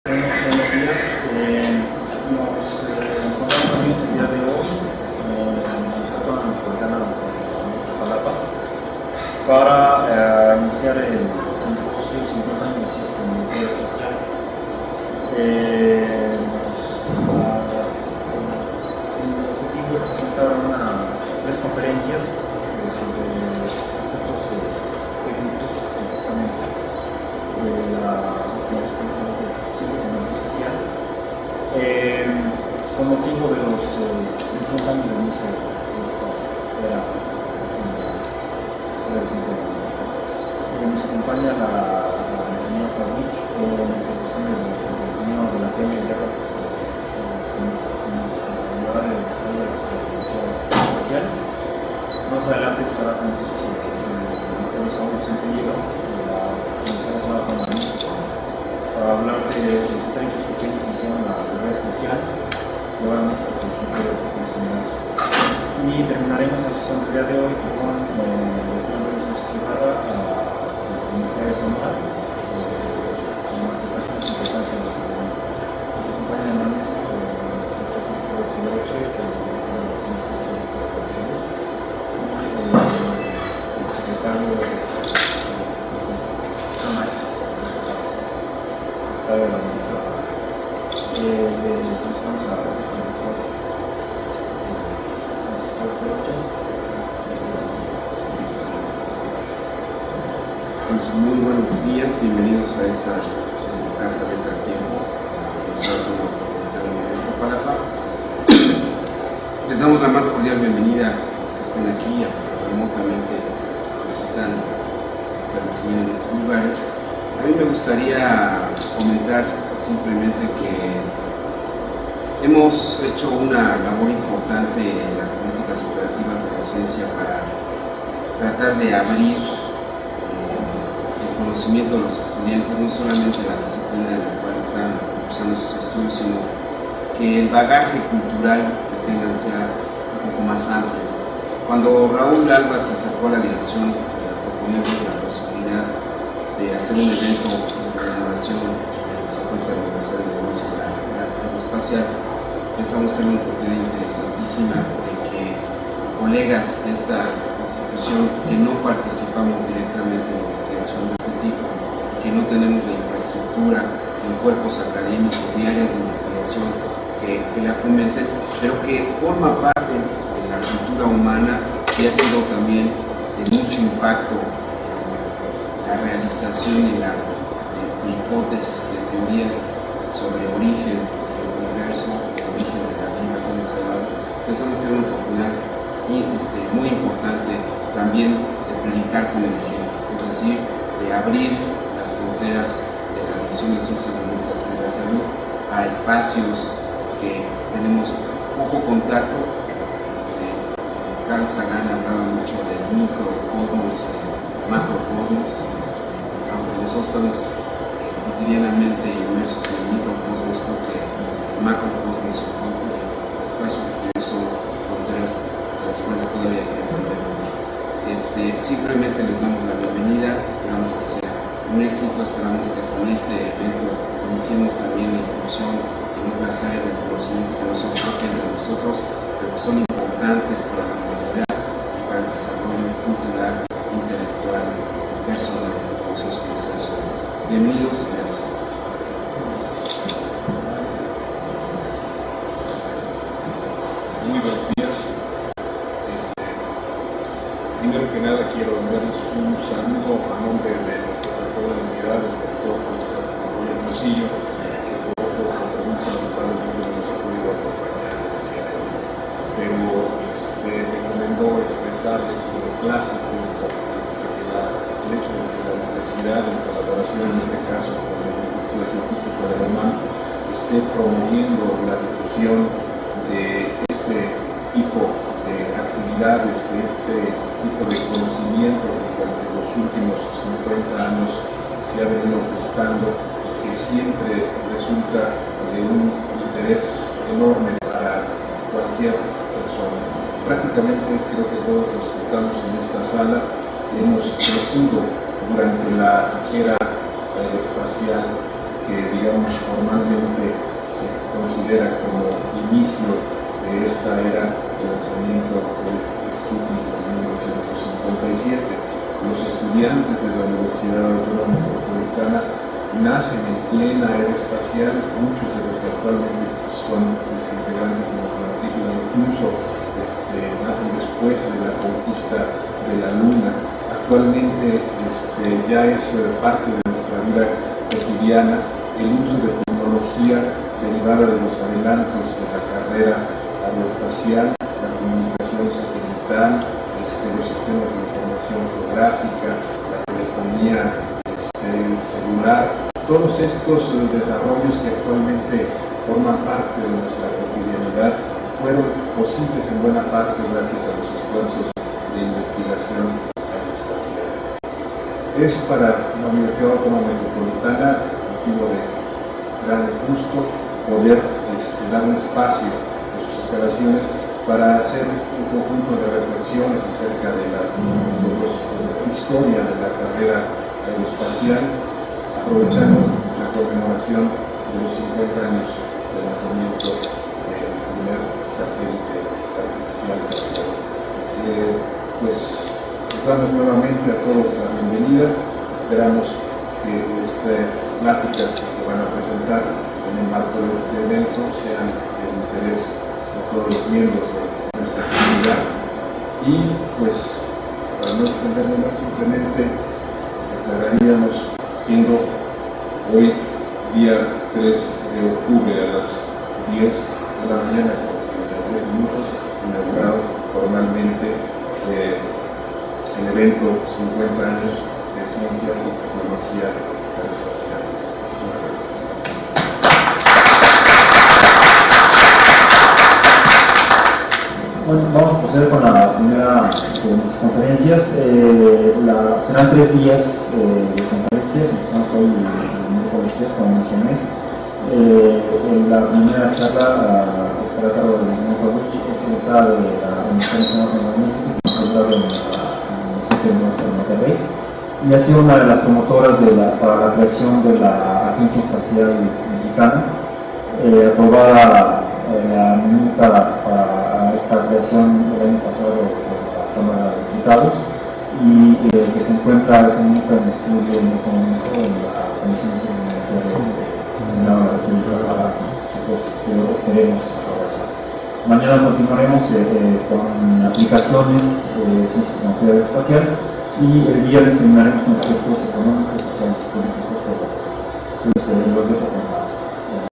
50 Años de Ciencia y Tecnología Aeroespacial - UAM Octubre, 2007
50 Años de Ciencia y Tecnología Aeroespacial
Presentación
Dr Francisco Flores Pedroche, Rector de la Unidad Iztapalapa
Dr Roberto Torres Orozco, Secretario de la Unidad Iztapalapa
Dr Raúl Alva García, Organizador
MAT Lourdes L Cahuich, Ponente de la primera conferencia
Conferencias sobre la ciencia y tecnología aeroespacial en México, su estado, aplicaciones, marco legal y la sociedad
Unidad Iztapalapa
Del 3 al 5 de octubre de 2007
Año Heliofísico Internacional 2007